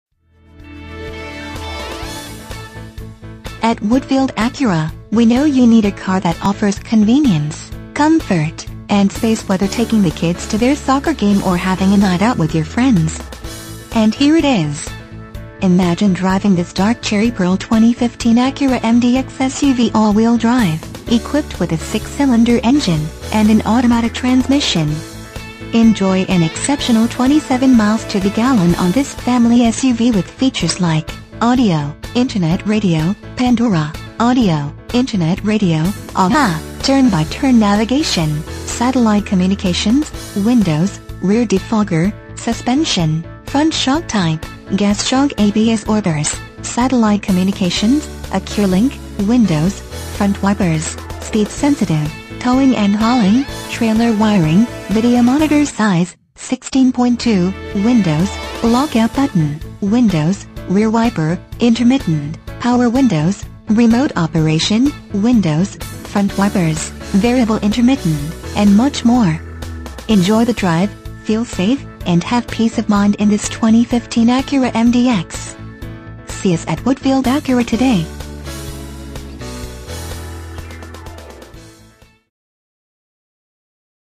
2015 Acura MDX Woodfield Acura Hoffman Estates, IL 60195
Acura MDX At Woodfield Acura, we know you are looking for a vehicle to solve everyday task. Whether loading the kids soccer equipment or having a night out with your friends. You will have the room you need making those everyday task an ease to accomplish. Imagine driving this Dark Cherry Pearl Acura Mdx SUV AWD, equipped with a 6 Cylinder engine and an automatic transmission.
MILES:  3,797
VIN:5FRYD4H85FB026272

Woodfield Acura
1099 w. Higgins Rd.